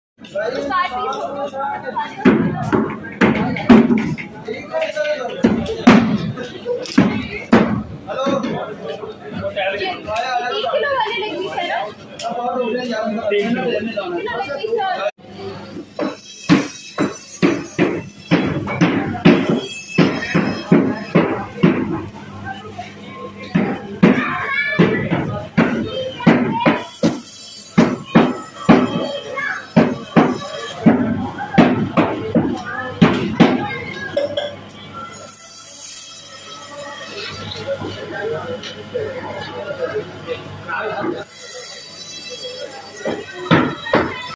Chicken🐔,Beef🍖 & Mutton|| @artwithpareezykhan
live Chicken beef and mutton shop in super market in pakistan
there we found fresh meat.